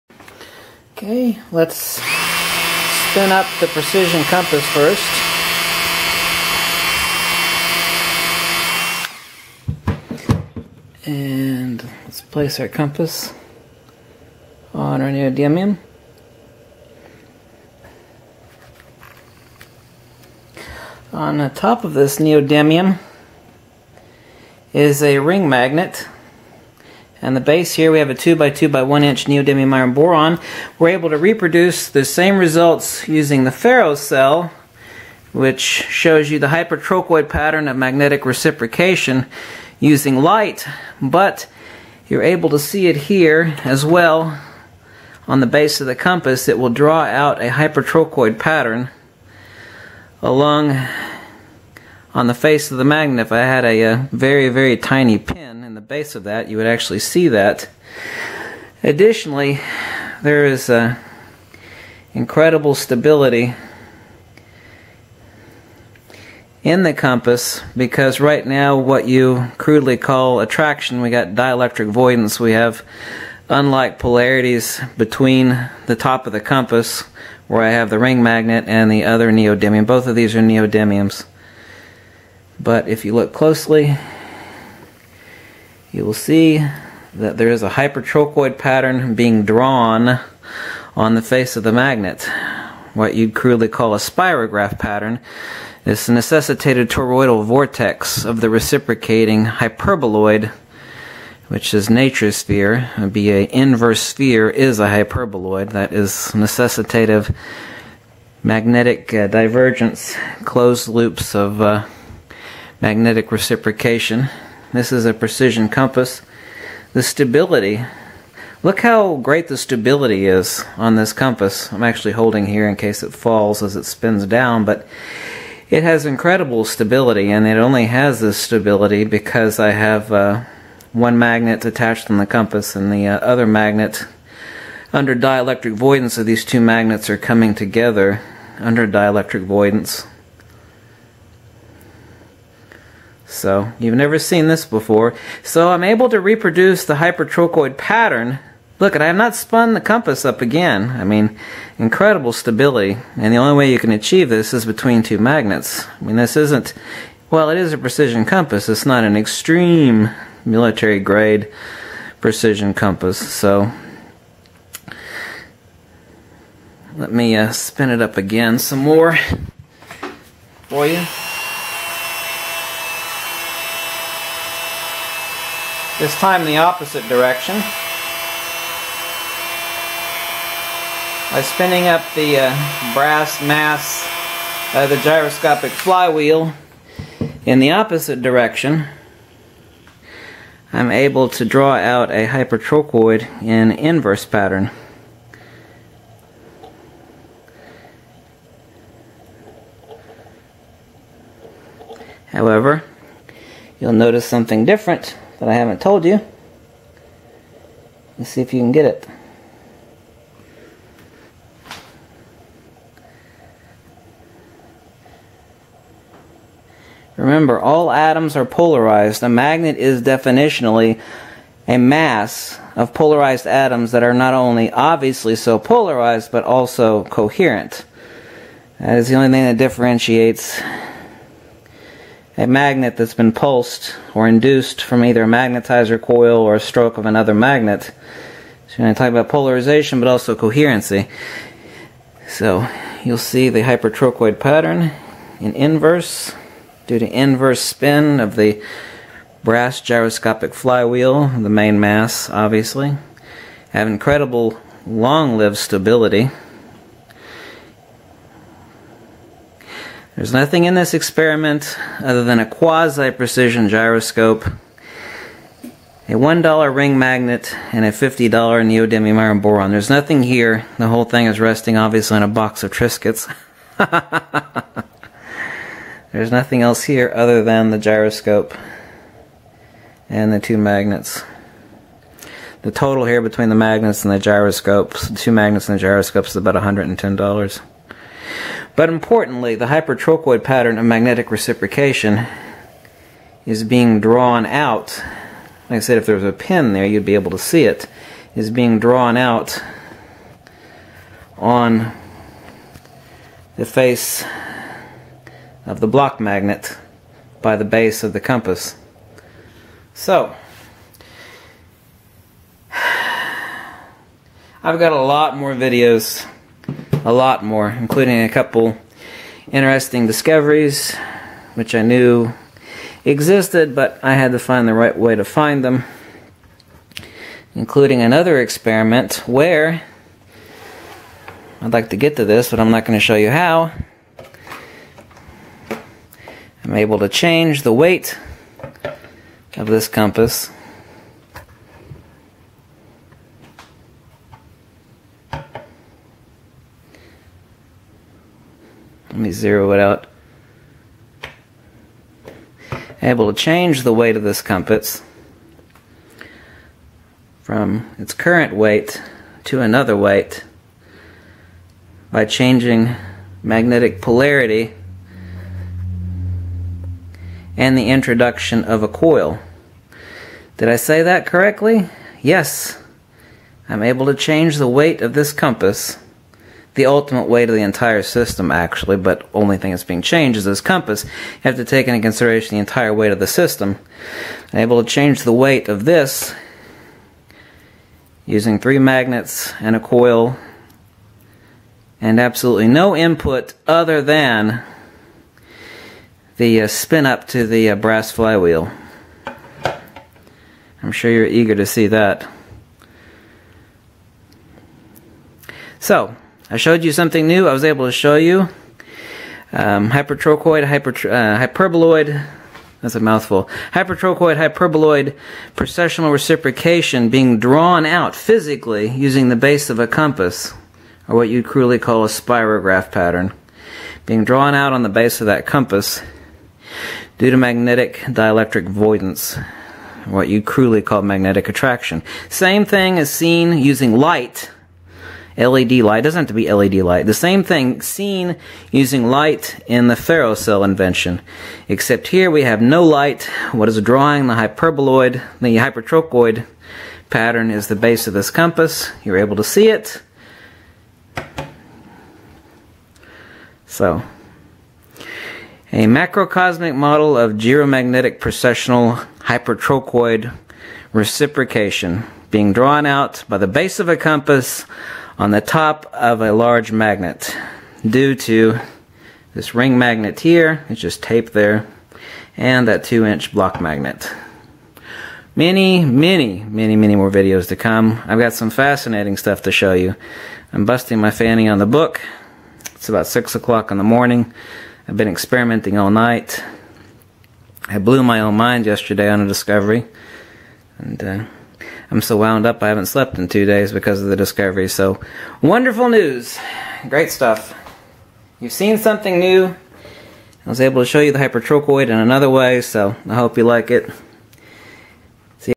VIDEO 110 UNCOVERING SECRETS OF MAGNETISM. A Gyroscope to see Hypotrochoid Magnetic reciprocation
VIDEO 110 UNCOVERING SECRETS OF MAGNETISM. Using a Gyroscope to see Hypotrochoid Magnetic reciprocation.

Likewise incredible gyroscopic stability due to dielectric voidance.